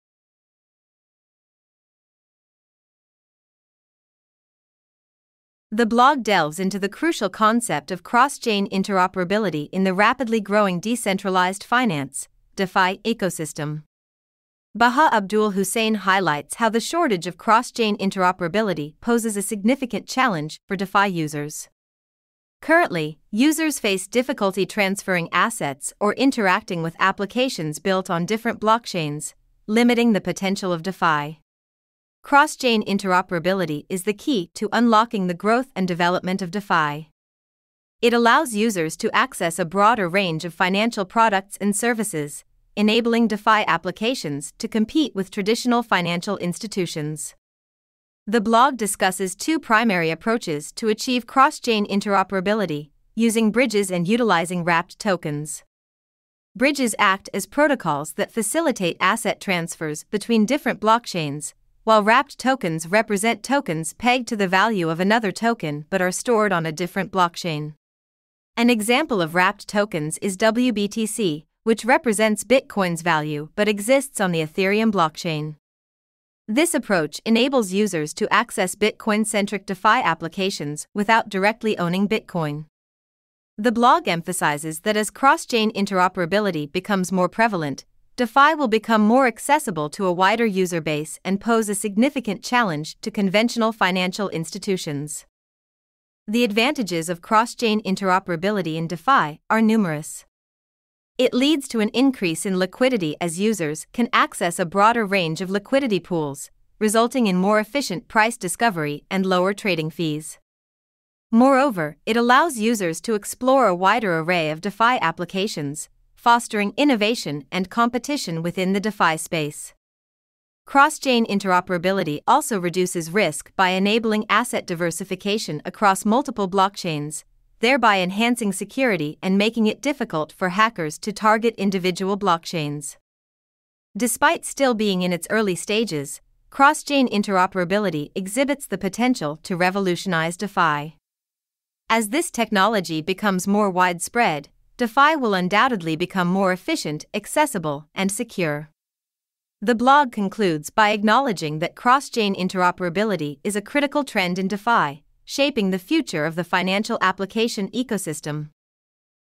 Cross-Chain Interoperability in DeFi | Bahaa Abdul Hussein
Unlocking DeFi's Potential: Cross-chain interoperability is the game-changer! Explore the benefits and challenges as blockchain technology bridges the gap.